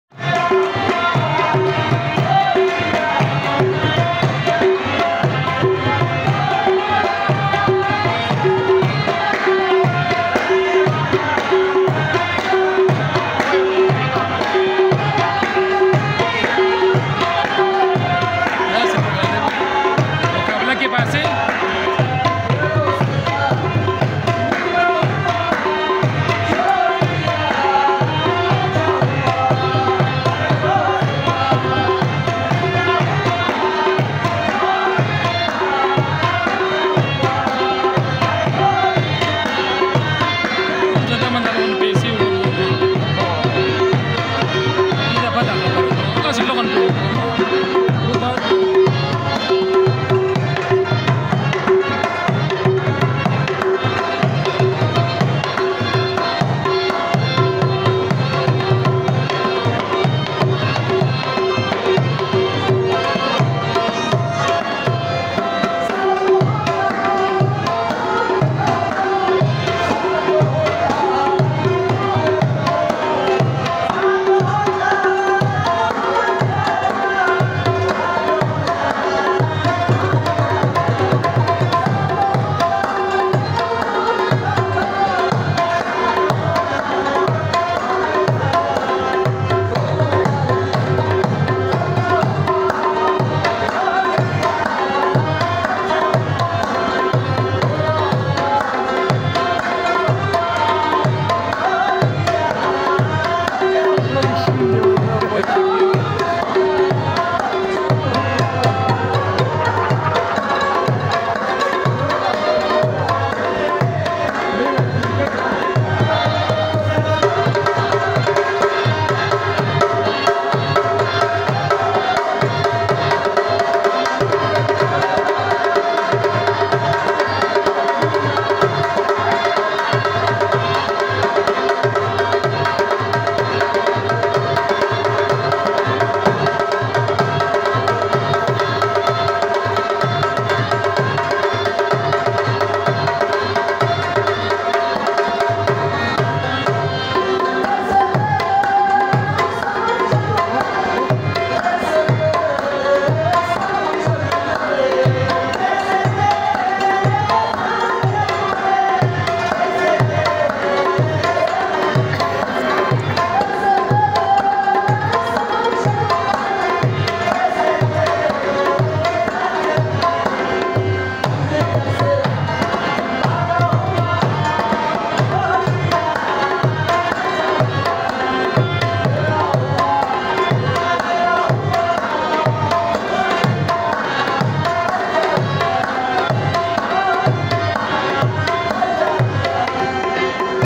mast New 2024 dance hazara gudwalian program Gul Akbar Rahi aor Arshad Akbar
مست ڈانس ہزارہ 2024 گدوالیاں پروگرام سنگر گل اکبر راہی اور ارشد اکبر